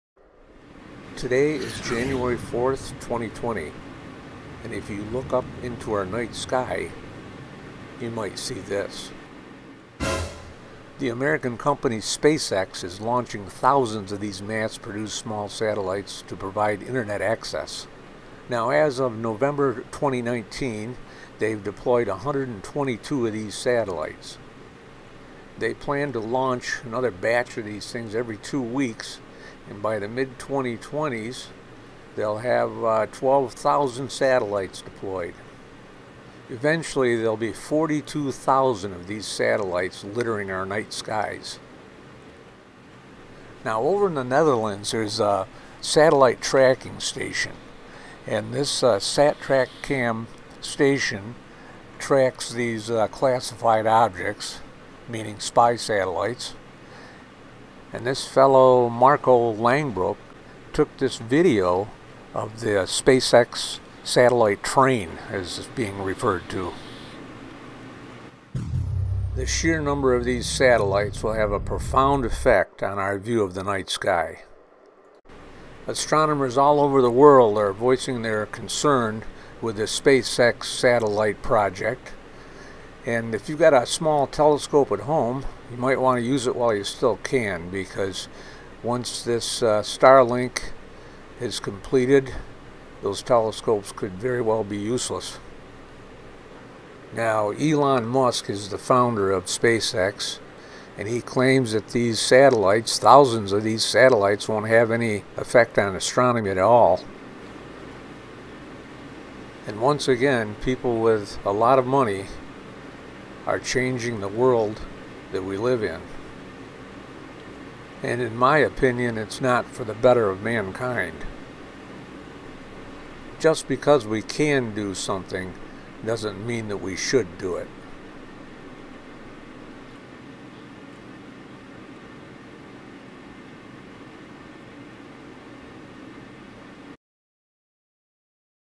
Starlink Satellite Project Concerns Astronomers Worldwide
SpaceX is launching thousands of satellites to provide internet service.  These satellites will have a profound affect on our view of the night sky and have astronomers worldwide voicing their concerns.